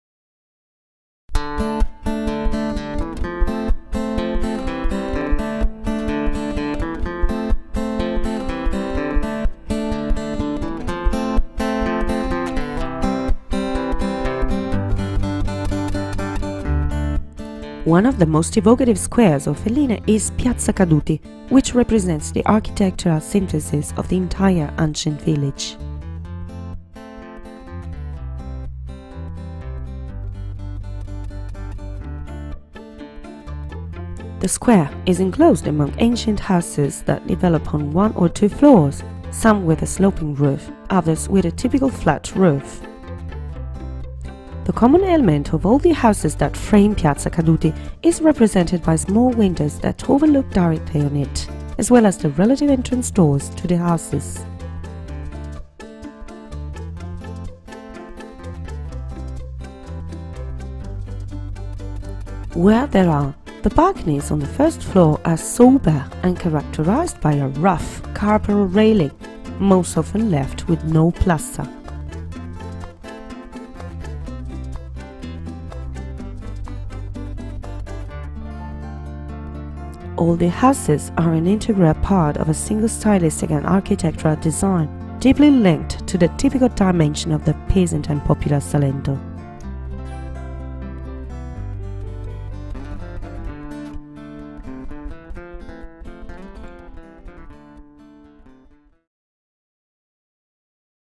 City of Alliste - Piazza Caduti – Square of Fallen Soldiers
One of the most evocative squares of Felline is Piazza Caduti, which represents the architectural synthesis of the entire ancient village.
The square is enclosed among ancient houses that develop on one or two floors, some with a sloping roof, others with a typical flat roof.
The common element of all the houses that frame Piazza Caduti is represented by small windows that overlook directly on it, as well as the relative entrance doors to the houses.